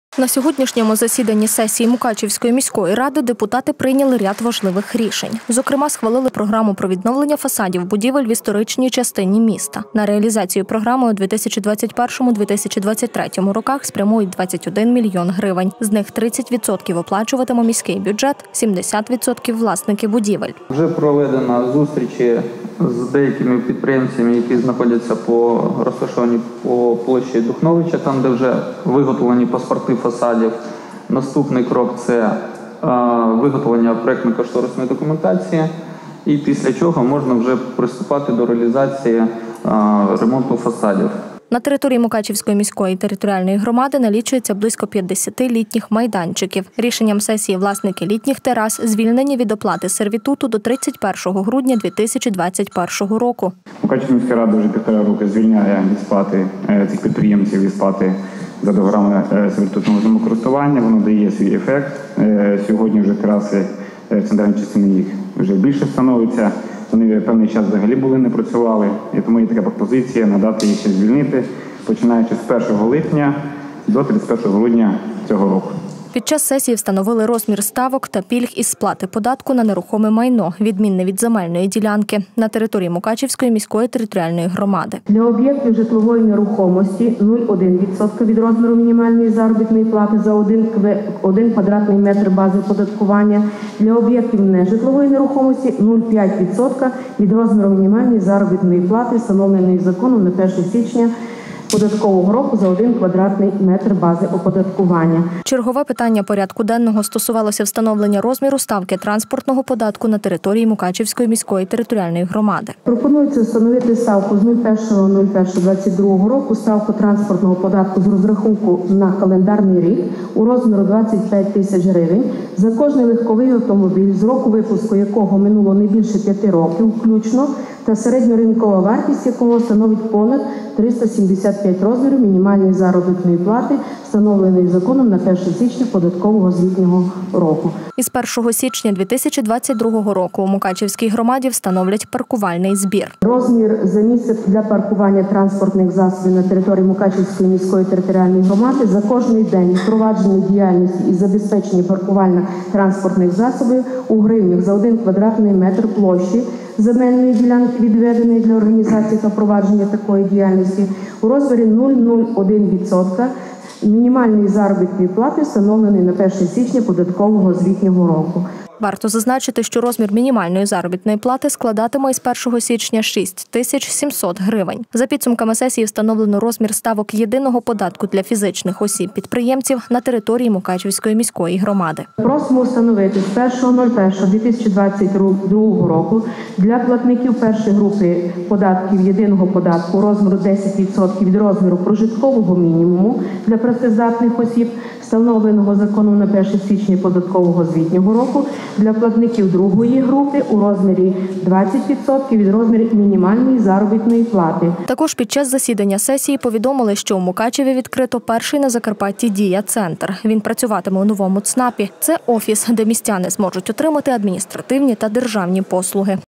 Сесія міської ради. Прийняті рішення
На сьогоднішньому засіданні сесії Мукачівської міської ради депутати прийняли ряд важливих рішень. Зокрема схвалили програму про відновлення фасадів будівель в історичній частині міста. На реалізацію програми у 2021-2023 роках спрямують 21 мільйон гривень, з них 30% оплачуватиме міський бюджет, 70% – власники будівель. На території Мукачівської міської ТГ налічується близько 50 літніх майданчиків. Рішенням сесії власники літніх терас звільненні від оплати сервітуту до 31 грудня  2021 року.Під час сесії встановили розмір ставок та пільг із сплати податку на нерухоме майно, відмінне від земельної ділянки, на території Мукачівської міської ТГ.Чергове питання порядку денного стосувалося встановлення розміру ставки транспортного податку на території Мукачівської міської ТГ.Варто зазначити, що розмір мінімальної заробітної плати складатиме з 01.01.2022 року - 6700 грн. За підсумками сесії встановлено розмір ставок єдиного податку для фізичних осіб – підприємців на території Мукачівської міської ТГ.Також під час засідання сесії повідомили, що в Мукачеві відкрито перший на Закарпатті Дія Центр. Він працюватиме в новому ЦНАПі. Це офіс, де містяни зможуть отримати адміністративні та державні послуги.